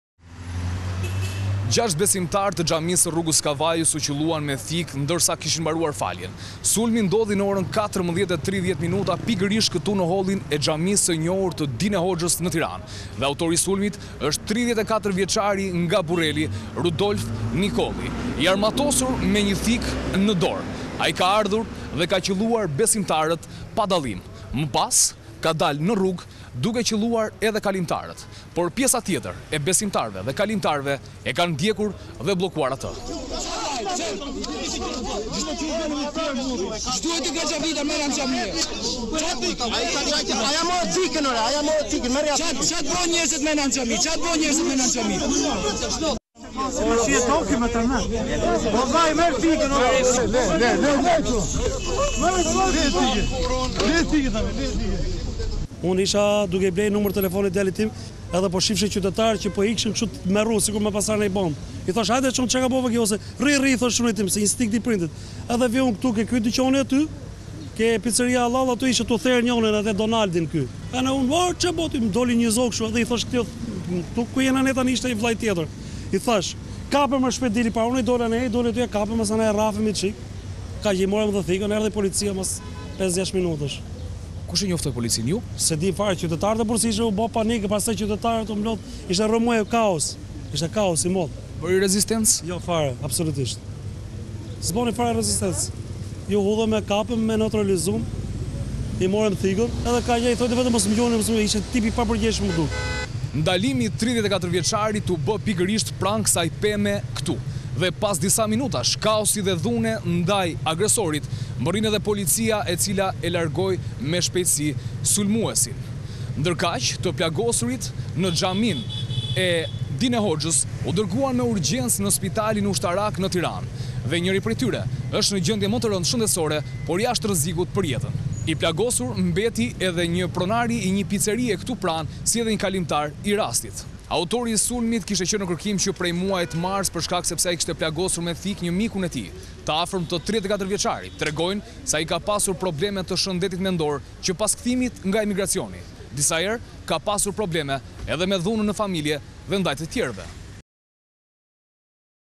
Top Channel/ Sulm me thikë në xhami/ Rudolf Nikolli qëlloi mbi besimtarët
Rreth orës 14:30, në Rrugën e Kavajës, një shtetas është futur në ambientet xhamisë “Dine Hoxha” ne Tiranë dhe në rrethana ende të paqarta ka plagosur me mjet prerës 5 shtetas.

Falë reagimit profesional dhe të menjëhershëm të shërbimeve të Policisë është bërë i mundur neutralizimi dhe prangosja e autorit të dyshuar, shtetasi Rudolf Nikolli 34 vjeç, nga Burreli, i cili po shoqërohet në ambientet e Drejtorisë Vendore të Policisë Tiranë, për veprime të mëtejshme.

Ndërkohë, shërbimet e Policisë iu kanë dhënë ndihmën e parë shtetasve të lënduar, të cilët janë transportuar në spital, për ndihmë mjekësore dhe ndodhen jashtë rrezikut për jetën.

Në vendngjarje ka mbërritur grupi hetimor dhe nën drejtimin e Prokurorisë vijon puna për sqarimin e rrethanave dhe dokumentimin e plotë ligjor të ngjarjes.

Ne këtë edicion ne do kemi lidhje direkte me gazetaret tanë qe ndodhen ne vendin e ngjarjes.